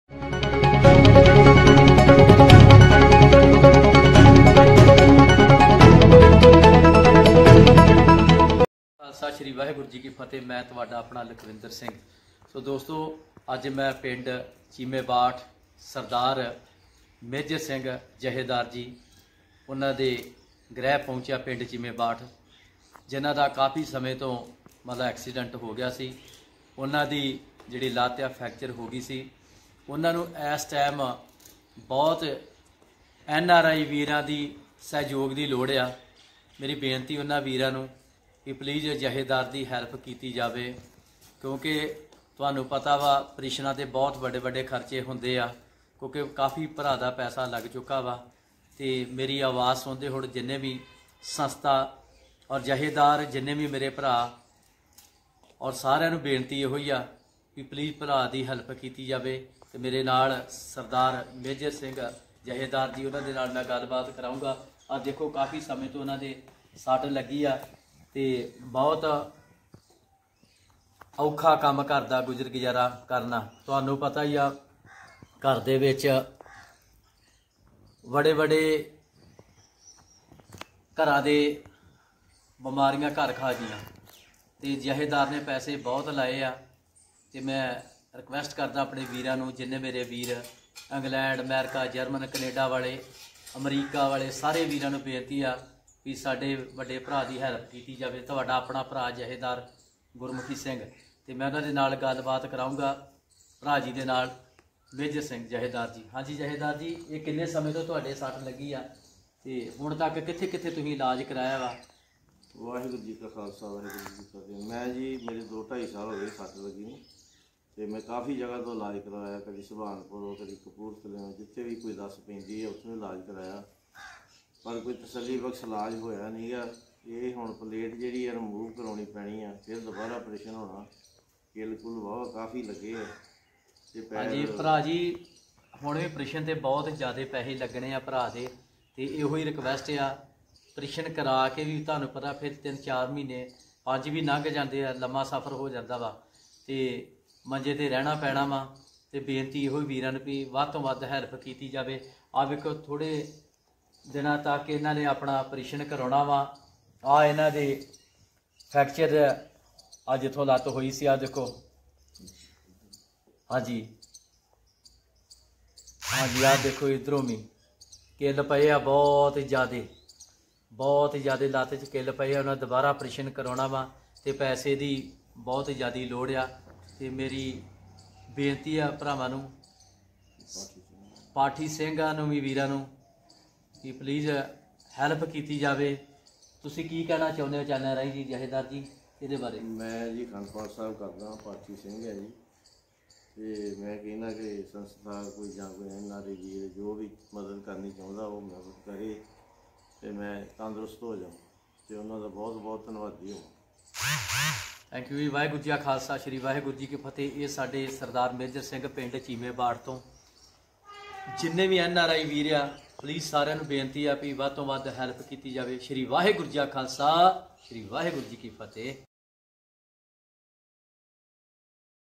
ਚੀਮਾਂ ਬਾਠ ਦੇ ਜਥੇਦਾਰ ਮੇਜਰ ਸਿੰਘ ਨੇ ਇਲਾਜ਼ ਲਈ ਐਨ ਆਰ ਆਈ ਵੀਰਾਂ, ਸਮਾਜ ਸੇਵੀ ਸੰਸਥਾਵਾਂ ਤੋਂ ਲਗਾਈ ਮੱਦਦ ਦੀ ਗੁਹਾਰ
Amritsar#cmbhagwantmaan#ktvpunjab#punjabpolice #